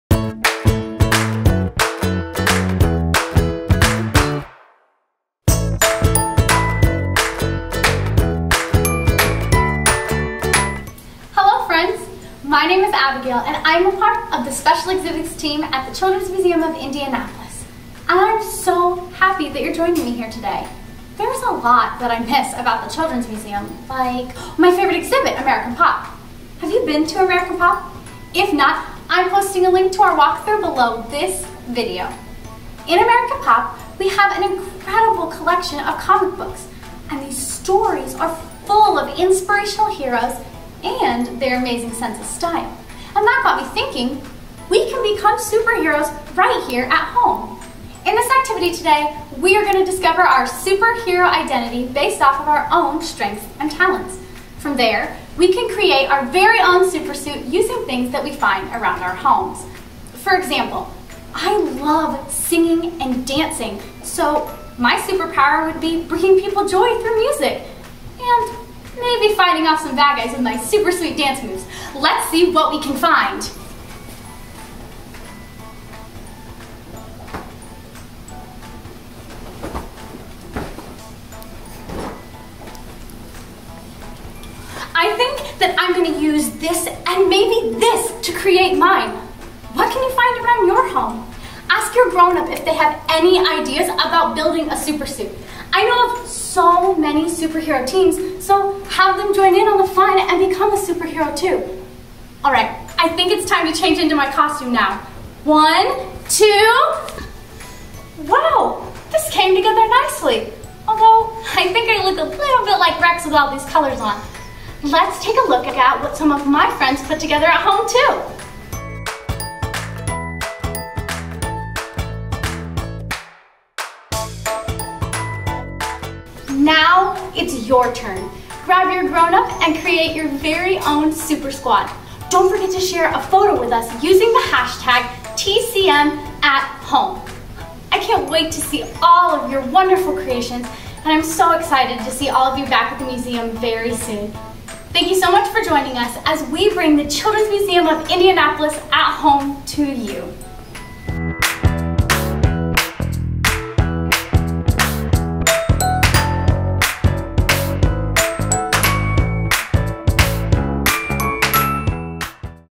Superhero Costume Challenge | Museum at Home
Be your own superhero! Find all kinds of things from around the house and dress up.